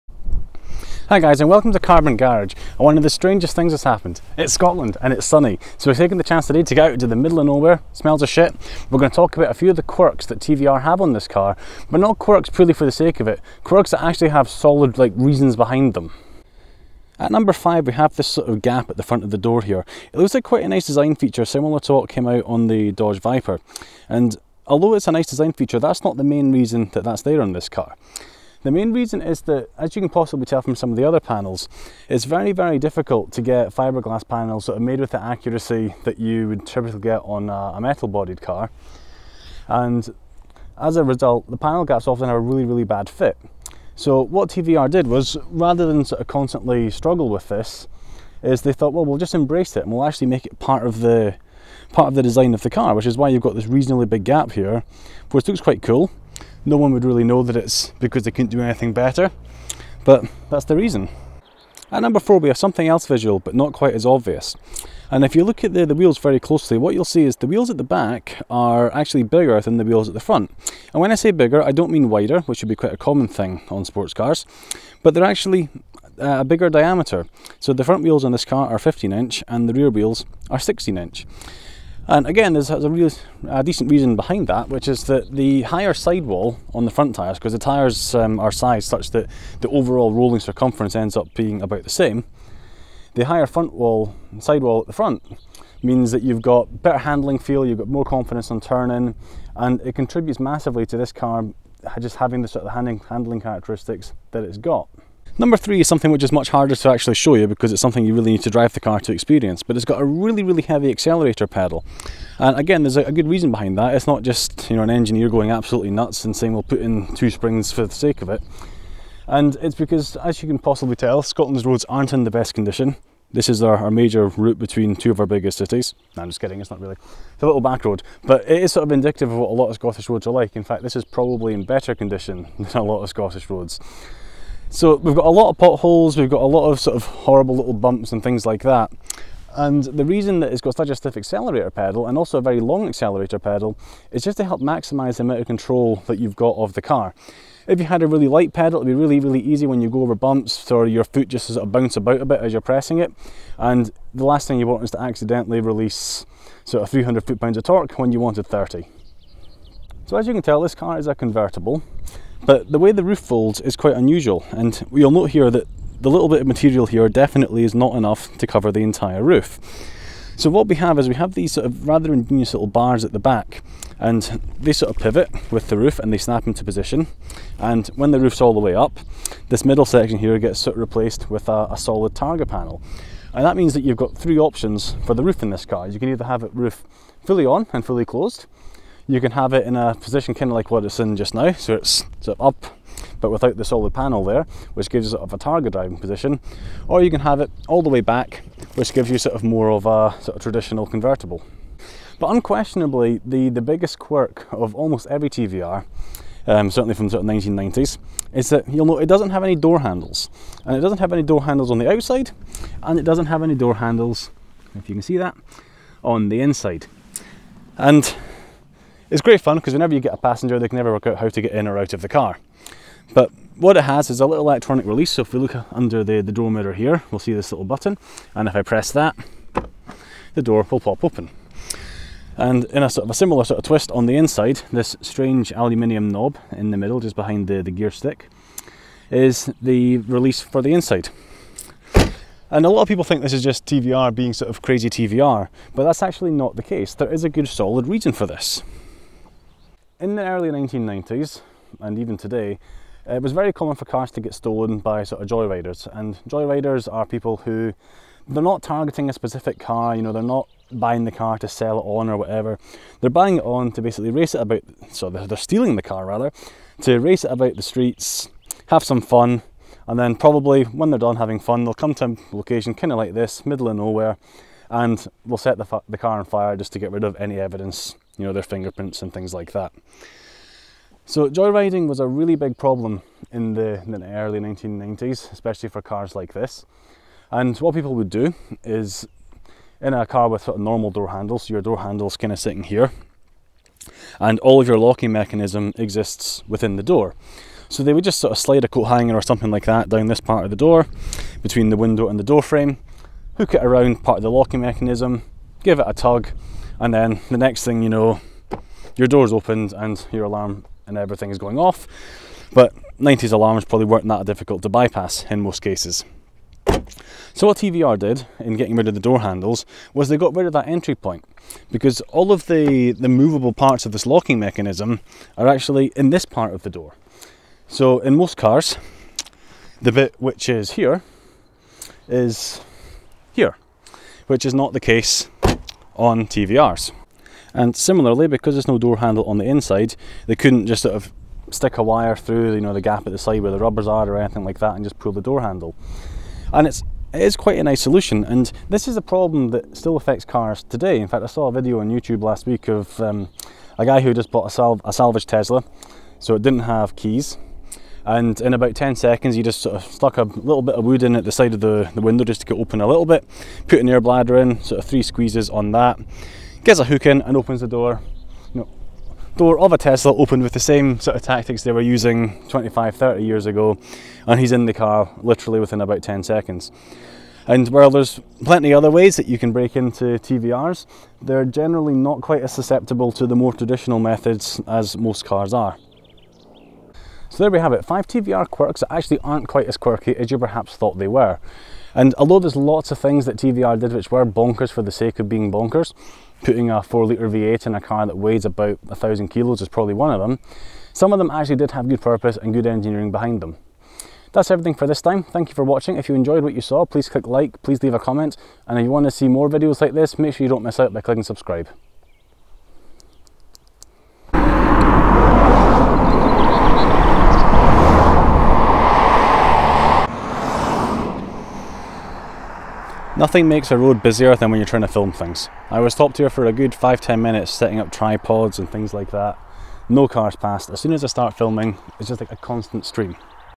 5 TVR quirks that have a purpose
TVR's are known for having some weird 'features' - like not having door handles - so in this video we look at five quirks that contrary to what you may think, are actually ingeniously solving actual problems.